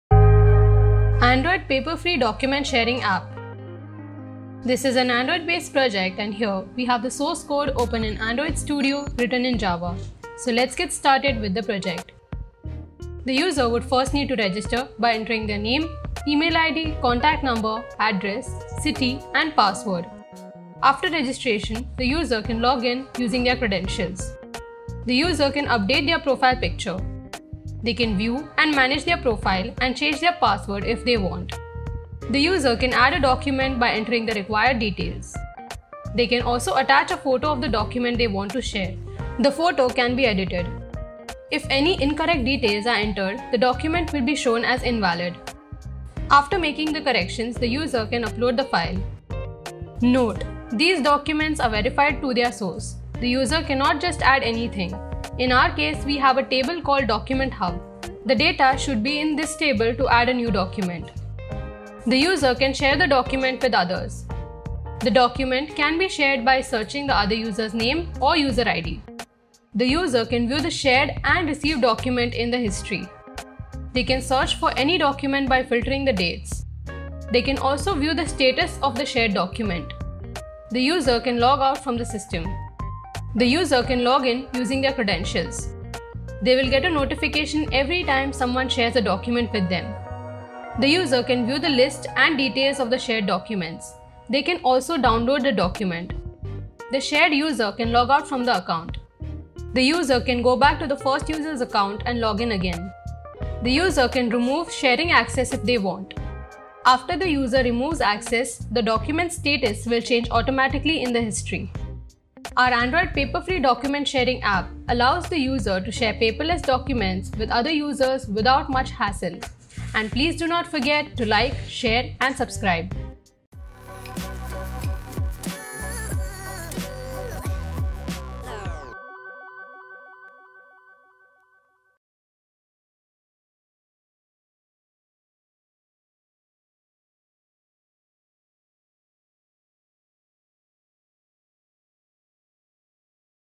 Android Paper Free Document Sharing App Project | Android App Project Ideas
Chopping down trees decreases the amount of oxygen in the ecosystem, thus adversely affecting all other life-forms. To avoid these circumstances and reduce the use of paper this android application can be used. In this application, User needs to register first with basic registration details and needs to create valid login credentials in order to access the application. After successful creation of account, users can login using valid credentials and access the application. There will be list of documents where user can Add, Update and Delete Documents. While Adding Documents, the Documents will be verified by the Certificate Parameters from the Database. Documents are captured and can be resized or Cropped to get the correct edges. Documents are Uploaded on the Server through FTP. Users can share documents to the users who are registered in the system by searching them by Name or Shared Id.